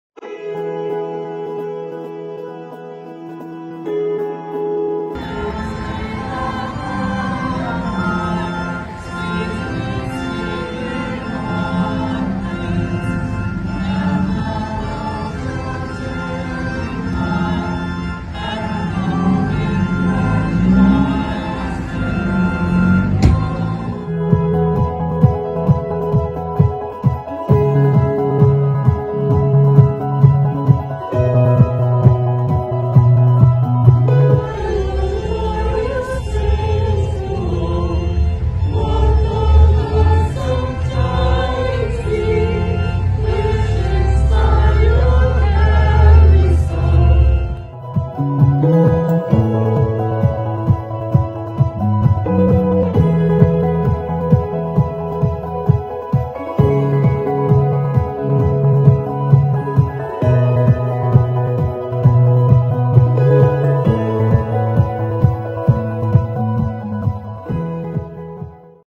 ABUMC - Caroling in the front yard of the Church
Algoma Blvd United Methodist Church - abumc
Pastor Cathy Weigand
Dec. 19, 2020 - 7:00 pm